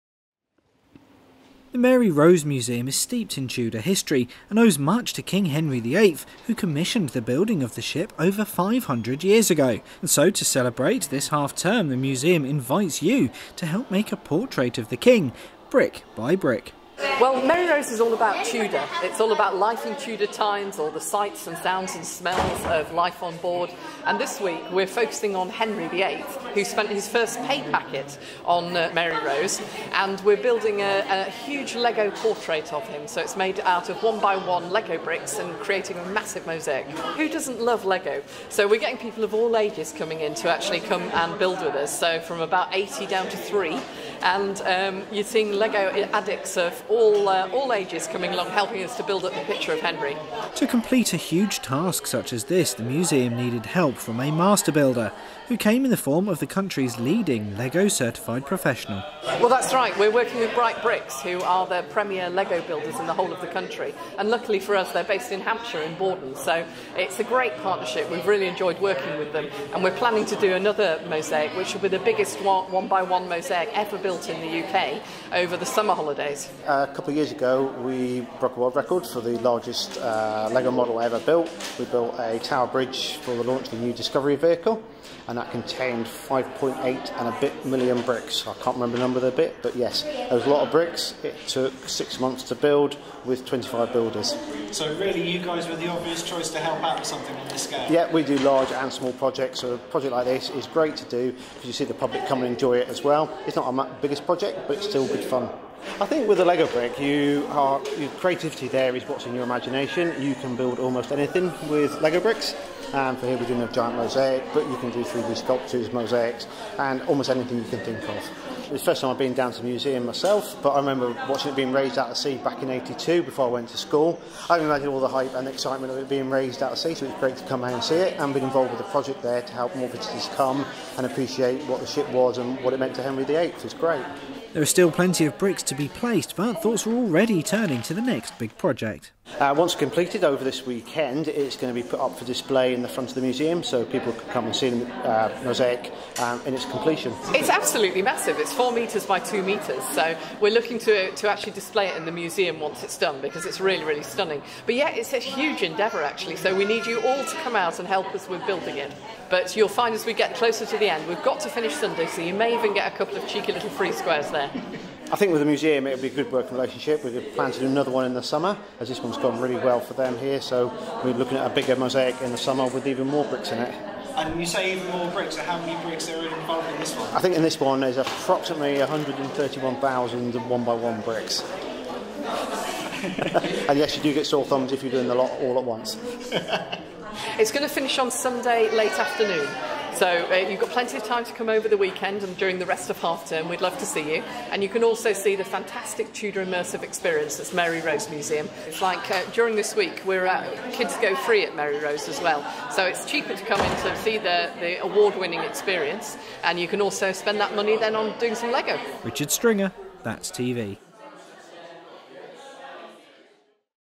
Thousands Of Lego Bricks Are Used To Build A Portrait Of Henry VIII
At some stage we have all played with it, and supposably there is enough out there for every person on the planet, to have over 60 pieces each.
 I am of course talking about LEGO.

And this half term, The Mary Rose museum is giving visitors the opportunity to help recreate a famous Tudor portrait of Henry VIII, using nothing but thousands of individual LEGO bricks!

The Project is In partnership with Bright Bricks, the UK’s only LEGO Certified Professional building company, and so our reporter Richard Stringer went to see how the portrait was coming along.